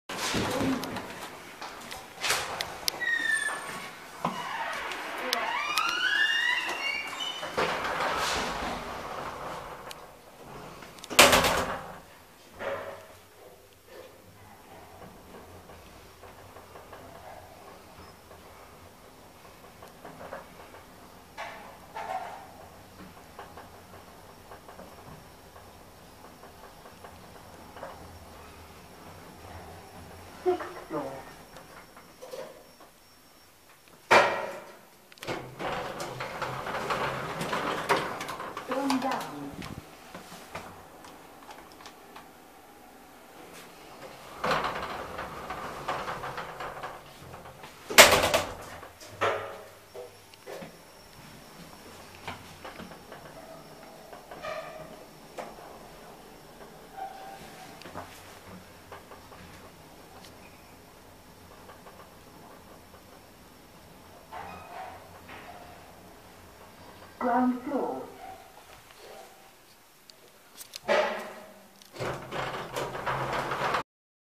Kone MonoSpace 500 MRL Traction Elevators at Ramada Hotel, Inn & Suites in Custom House, London
Nice elevators!

---Elevators---
Brand: Kone
Type: MRL Traction
Capacity: 630kg / 8 Persons
Model: MonoSpace 500 (Powered by EcoDisc)
Fixtures: Sigma
Floor served: 7(*0*, 1, 2, 3, 4, 5, 6)
Ministry of Industry, Trade & Labour numbers: 902970 & 902971